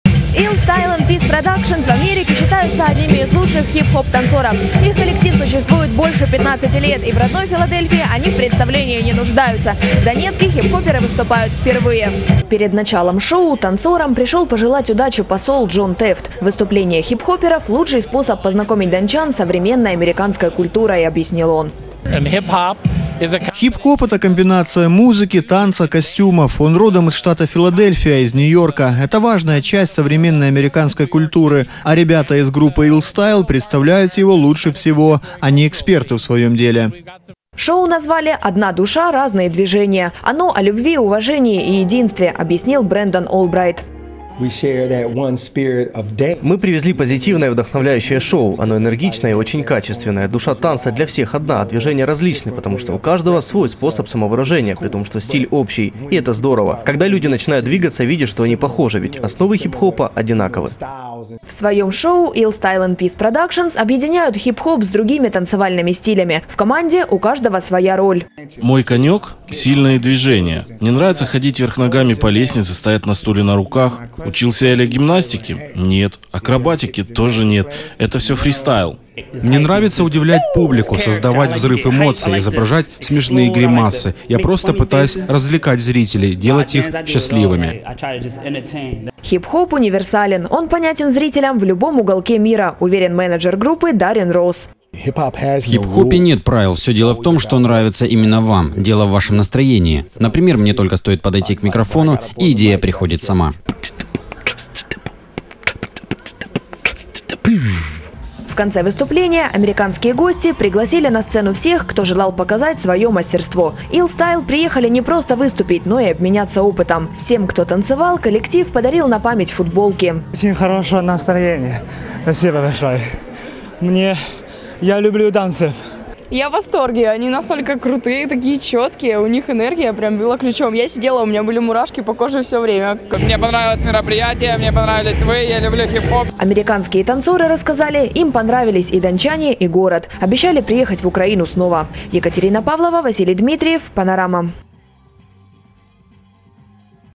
Дончанам показали хип-хоп
Дончанам показали настоящий хип-хоп. Профессиональные танцоры из штата Филадельфия выступили в шахтерской столице бесплатно. Их концерт проходил в рамках «Дней Америки в Донецке». На танцевальном шоу побывала и Екатерина Павлова